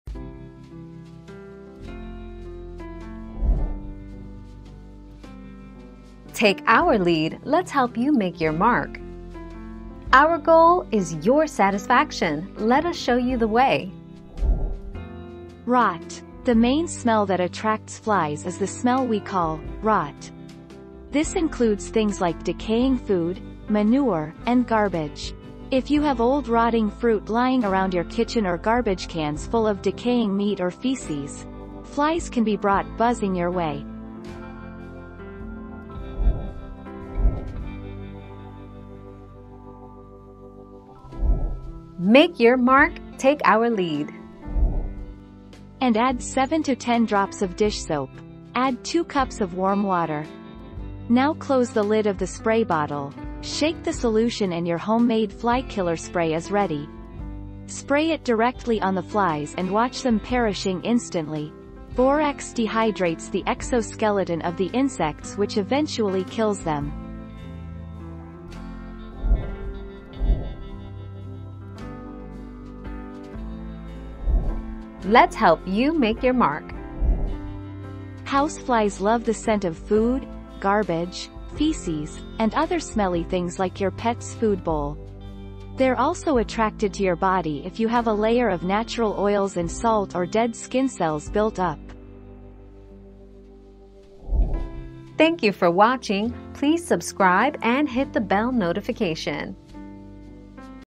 What smell attracts flies the most?
00:00 - What smell attracts flies the most?
00:38 - What can I spray on flies to kill them?
01:11 - Why do I attract flies?

Laura S. Harris (2021, June 4.) What smell attracts flies the most?
    AskAbout.video/articles/What-smell-attracts-flies-the-most-251072